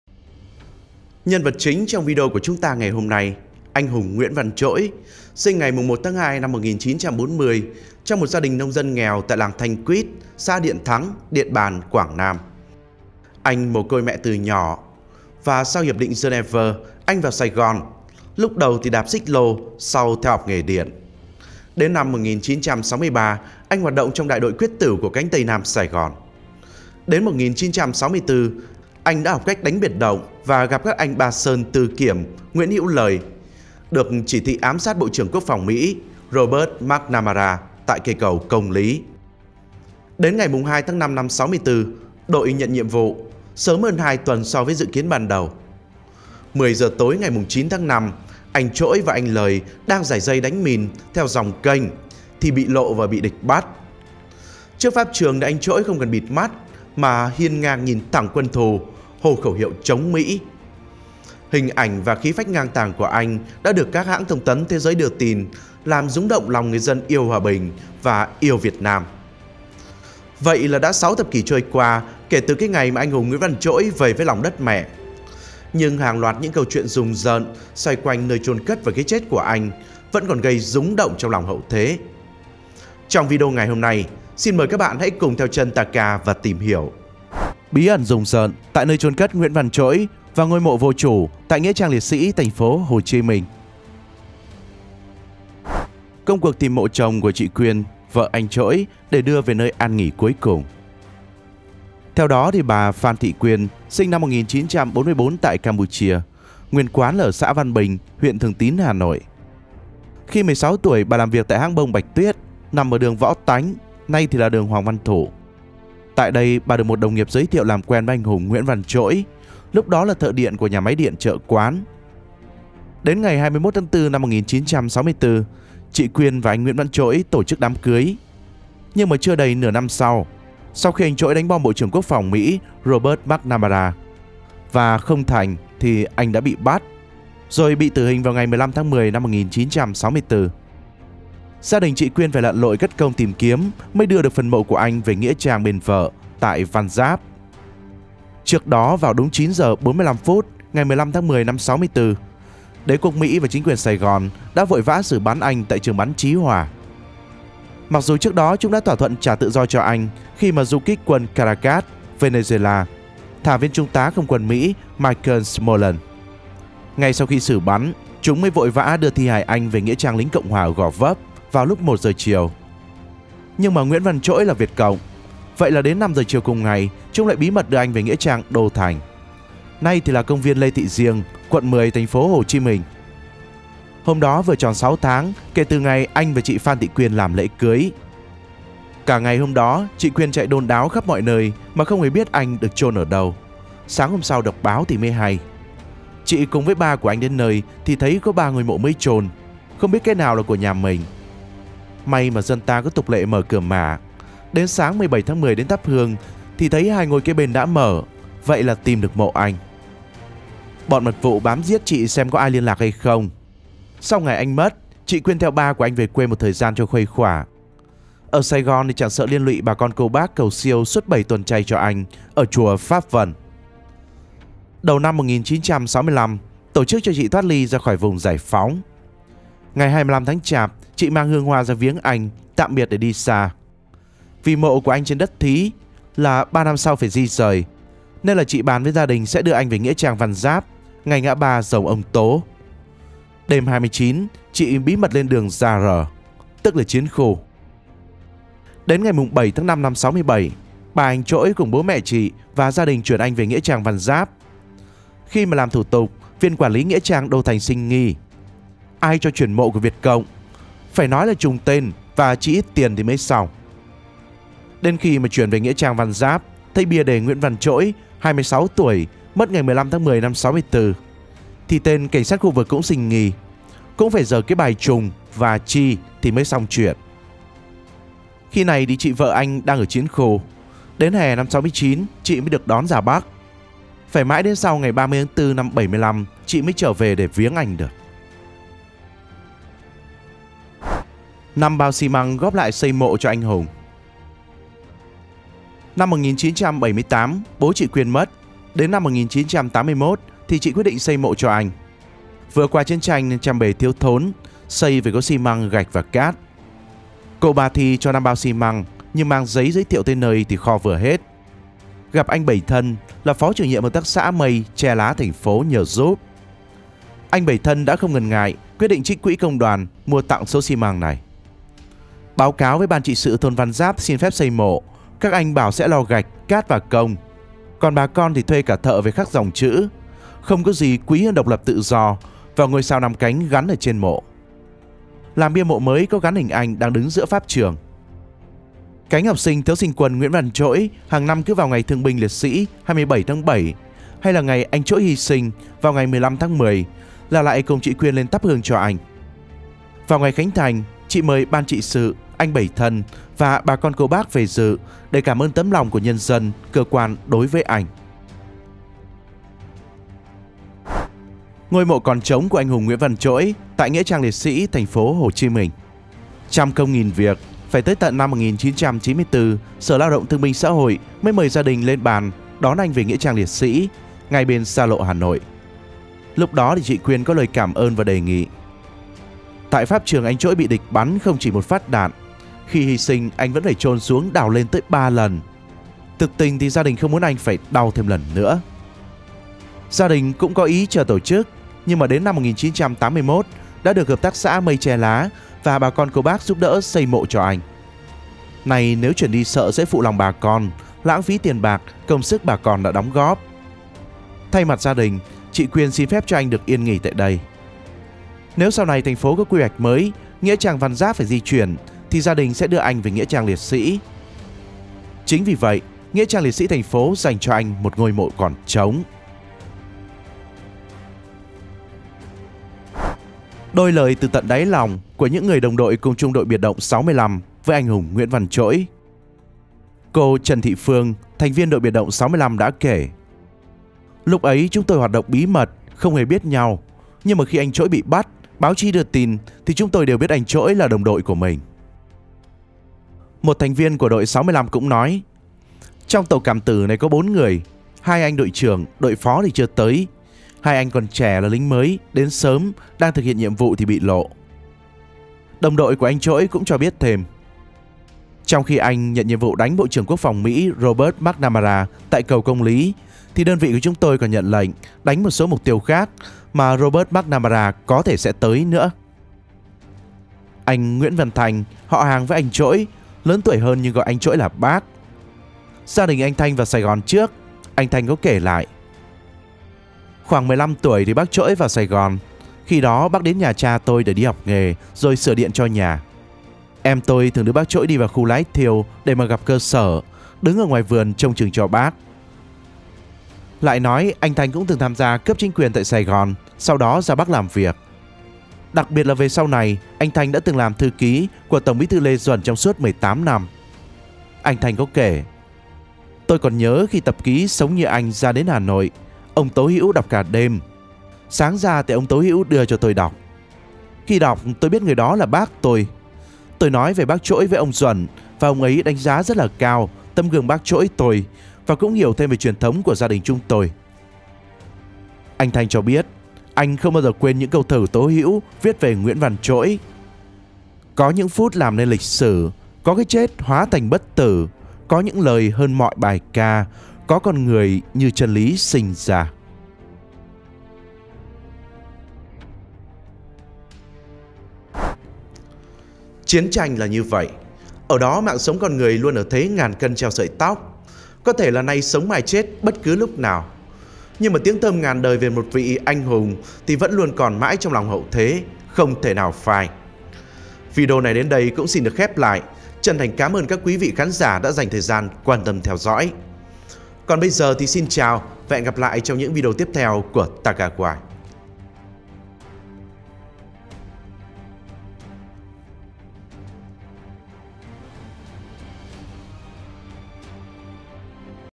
Bí Ẩn Về Cái Chết Rùng Rợn Của Nguyễn Văn Trỗi Và Ngôi Mộ Trống Tại Sài Gòn Đàm Đạo Lịch Sử #104
Taca Why xin mến chào các bạn khán giả yêu quý! Nhân vật chính trong video của chúng ta ngày hôm nay, anh Nguyễn Văn Trỗi - sinh ngày 1/2/1940 trong một gia đình nông dân nghèo tại làng Thanh Quýt, xã Điện Thắng, Điện Bàn, Quảng Nam. Anh mồ côi mẹ từ nhỏ. Sau hiệp định Genève, anh vào Sài Gòn, lúc đầu đạp xích lô, sau theo học nghề điện. Đến năm 1963, anh hoạt động trong Đại đội quyết tử của cánh Tây Nam Sài Gòn. Năm 1964, anh ra cứ học cách đánh biệt động và gặp các anh Ba Sơn, Tư Kiếm, Nguyễn Hữu Lời, được chỉ thị ám sát Bộ trưởng Quốc phòng Mỹ Robert Mc Namara tại cầu Công Lý. Ngày 2/5/1964, đội nhận nhiệm vụ, sớm hơn 2 tuần so với dự kiến ban đầu. 10 giờ tối  9/5/1964, anh Trỗi và anh Lời đang rải dây đánh mìn theo dòng kênh thì bị lộ và bị địch bắt. Trước pháp trường, anh Trỗi không cần bịt mắt mà hiên ngang nhìn thẳng quân thù hô khẩu hiệu chống Mỹ. Hình ảnh và khí phách ngang tàng của anh đã được các hãng thông tấn thế giới đưa tin làm rung động lòng người dân yêu hòa bình và yêu Việt Nam. Vậy là đã gần 6 thập kỷ trôi qua kể từ ngày anh hùng Nguyễn Văn Trỗi về với lòng đất mẹ. Nhưng hàng loạt những câu chuyện rùng rợn xoay quanh nơi chôn cất anh vẫn còn gây rúng động trong lòng hậu thế. Trong video ngày hôm nay, xin mời các bạn cùng theo chân Taca tìm hiểu.